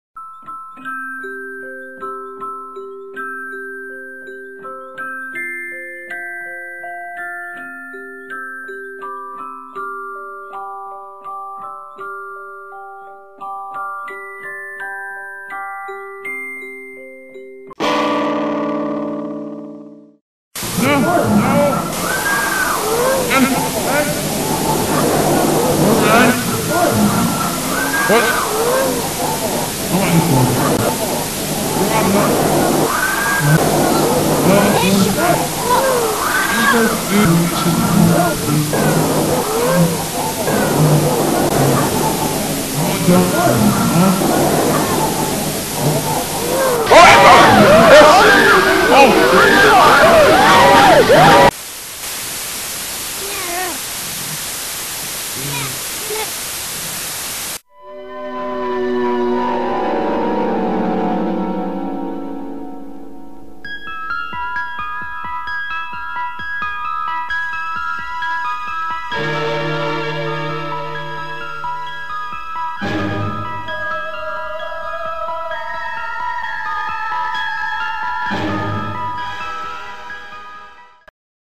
Toys FACES OF DEATH Episode 2 - Suicide!
Episode 2: a man hang himself. Little shockumentary-parody of Faces of Death.